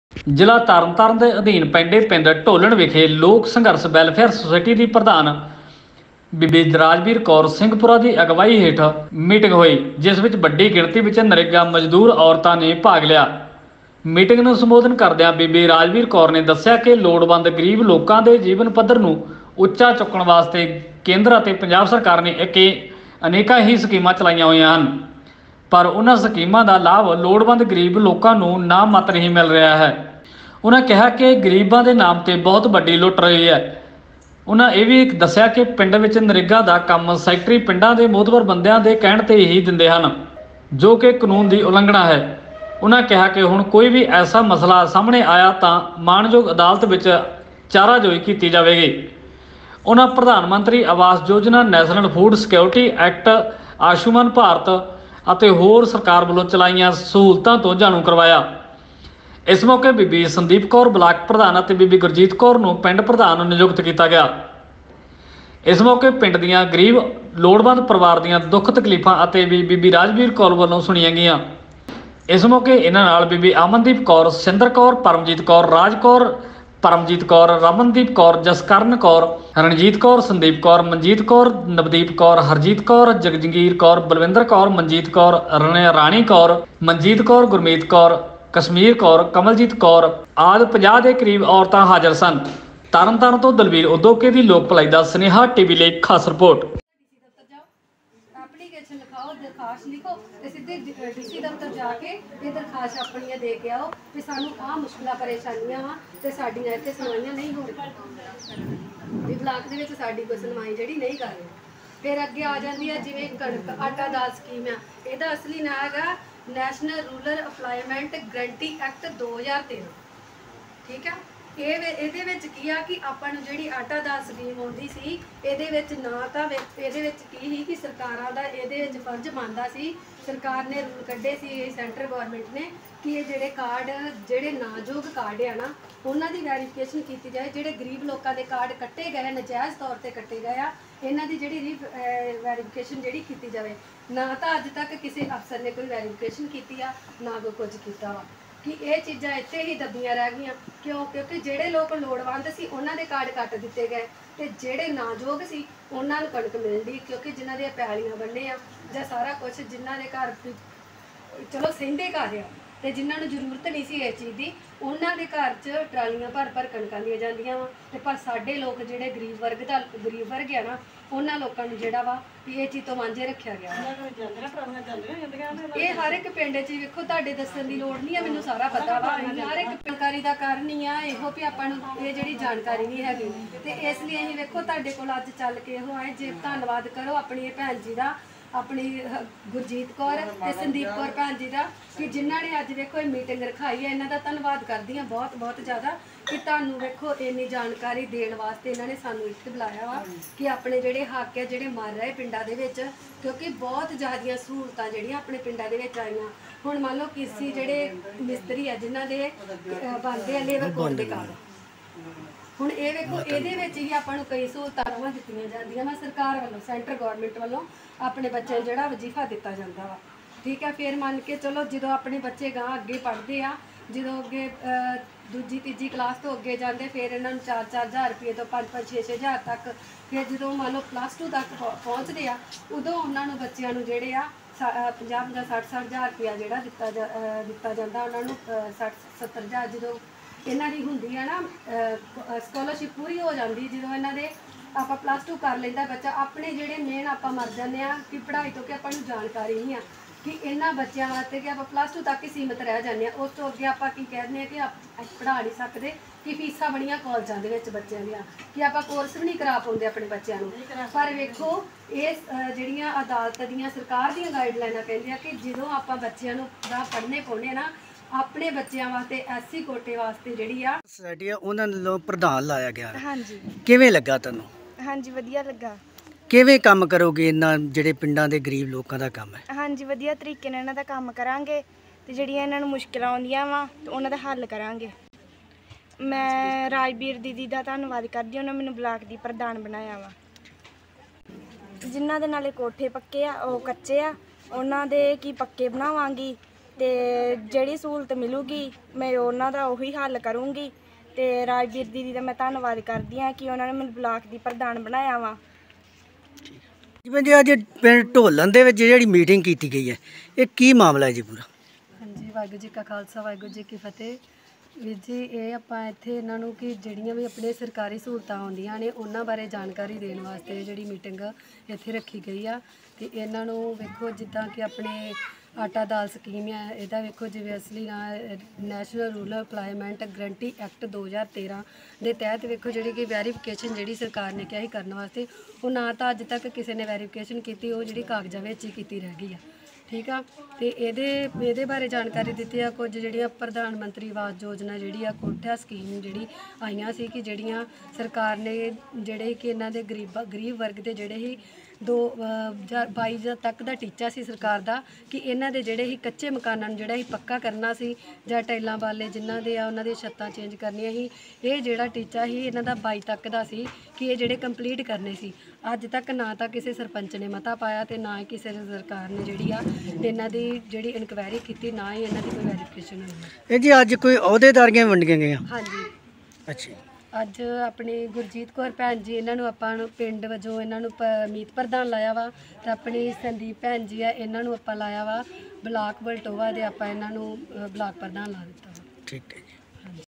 LBS NEWS LIVE || ਲੋਕ ਇਨਸਾਫ ਸਘਰੰਸ ਵੈਲਫੇਅਰ ਸੋਸੁਈਟੀ ਦੀ ਪਿੰਡ ਢੋਲਣ ਵਿਖੇ ਨਰੇਗਾ ਵਰਕਰਾਂ ਦੀ ਹੋਈ ਮੀਟਿੰਗ
Report By : ਲੋਕ  ਇਨਸਾਫ ਸਘਰੰਸ ਵੈਲਫੇਅਰ ਸੋਸੁਈਟੀ ਦੀ ਪੰਜਾਬ ਪ੍ਧਾਨ ਬੀਬੀ ਰਾਜਬੀਰ ਕੌਰ ਸਿੰਘਪੁਰਾ ਦੀ ਅਗਵਾਈ ਹੇਠ ਹੋਈ ਪਿੰਡ ਢੋਲਣ ਵਿਖੇ ਹੋਈ ਨਰੇਗਾ ਵਰਕਰਾਂ ਦੀ ਮੀਟਿੰਗ